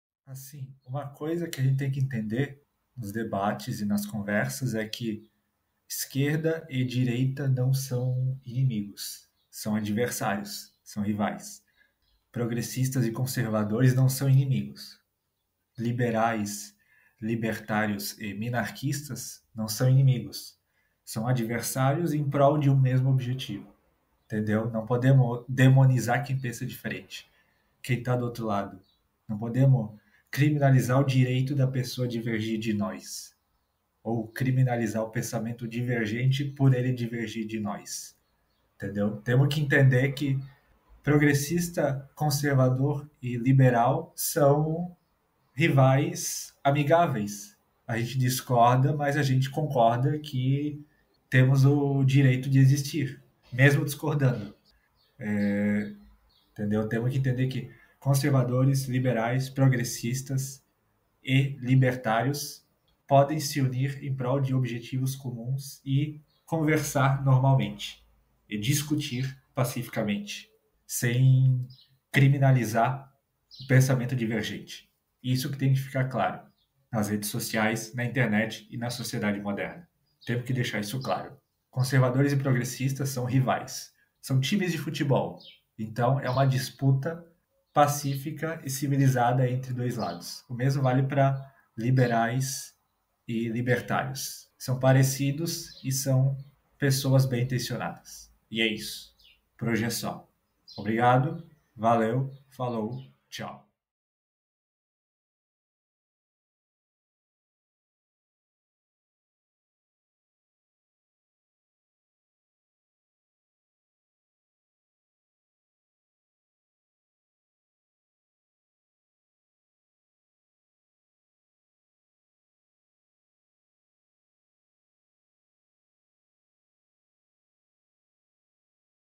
Vlog 225 - Os Progressistas VS Os Conservadores.
Nesse vídeo, eu falo sobre a disputa ideológica entre progressistas, conservadores, liberais e minarquistas no Brasil.

Refs.: Pixabay, Google Imagens, Google Drawings e Duck Duck Go.